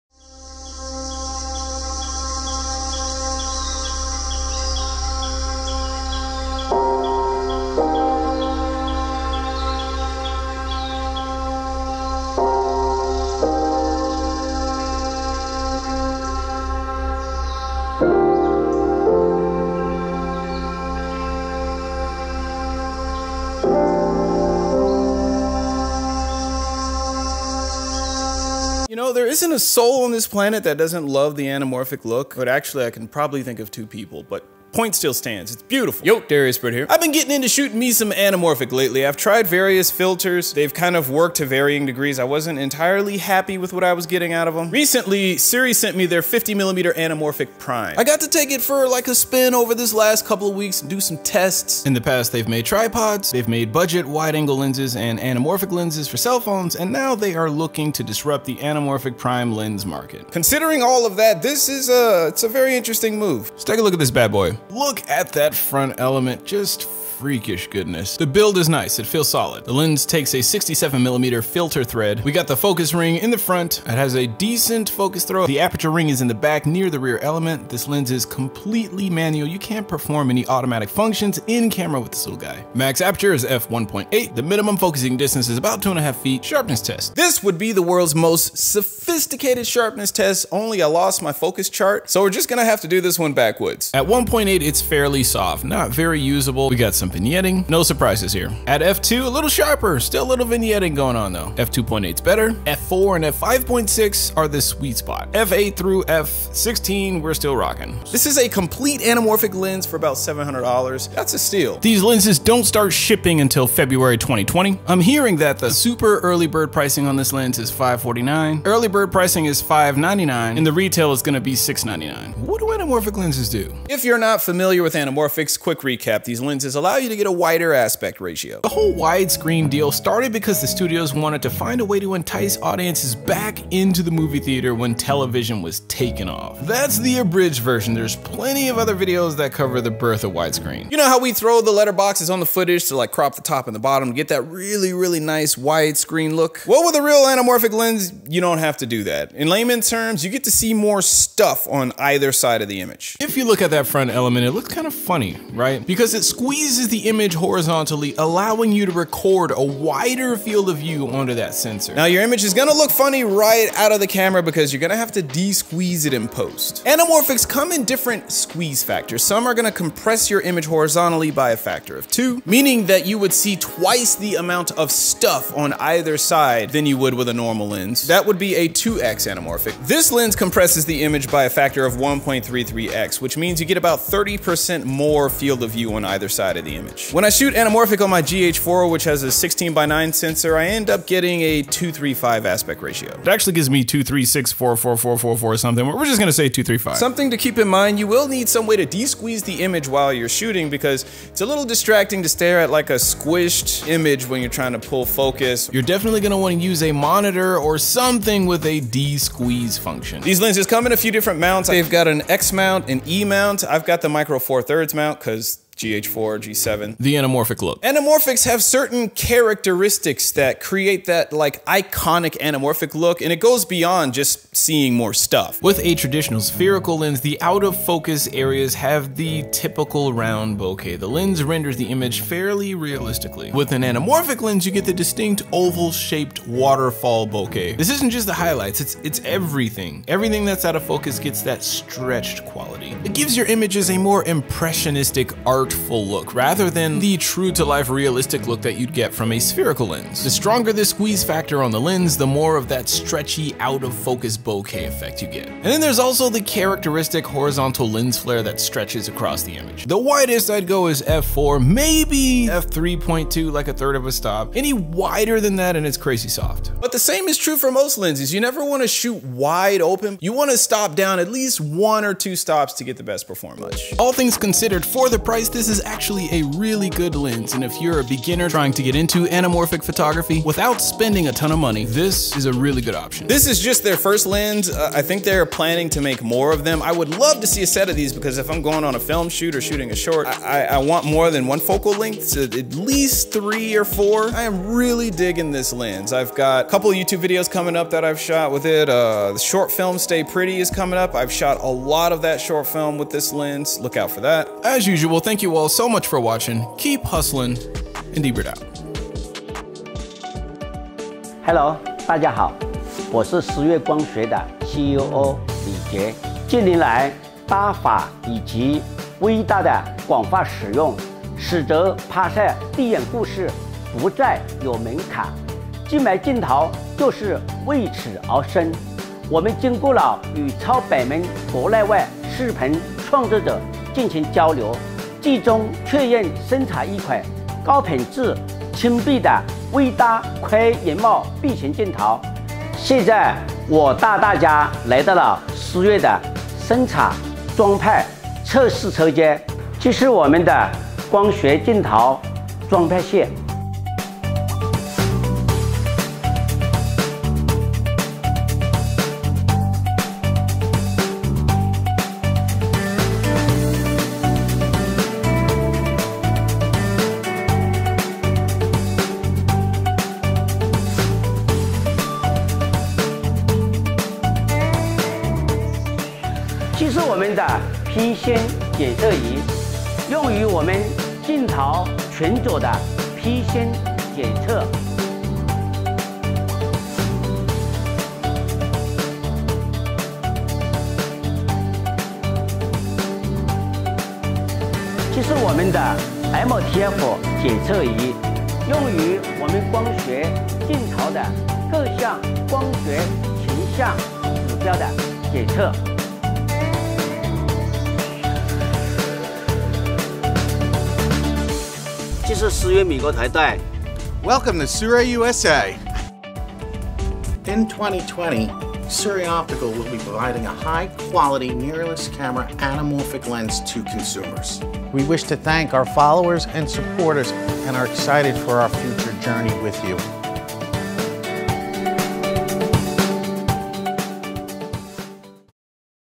How (well) the anamorphic works - Key Features of SIRUI 50mm anamorphic Lens
Take a look at how the anamorphic lens works and more bonus details!